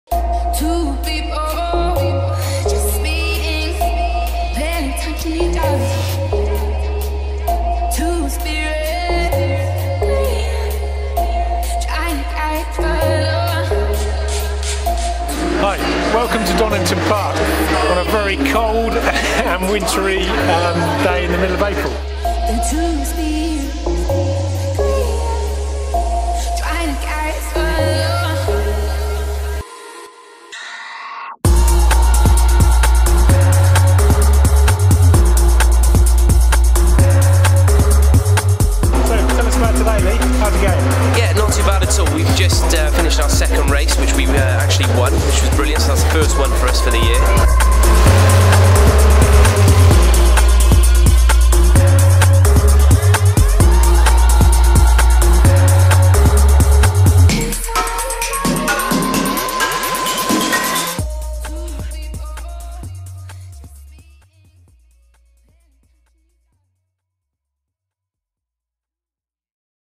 BTCC Short Promo
Short cut from the Donington round of the BTCC Produced by Broadcast City